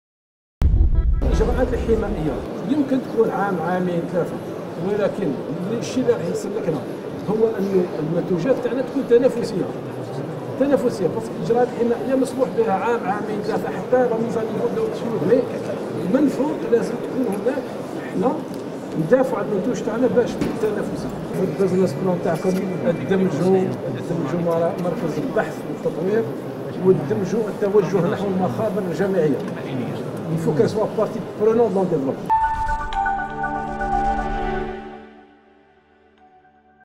الوزير الأول: ترقية المنتوج الوطني تمر عبر تحسين تنافسية و ليس عبر الإجراءات الحمائية
الجزائر (وأج)- أكد الوزير الاول،وزير المالية، أيمن بن عبد الرحمان،على ضرورة تحسين تنافسية المنتج الوطني من أجل التمكن من مواجهة المنتجات الاجنبية، بدل الاعتماد الكلي على الاجراءات الحمائية كونها لا تدوم .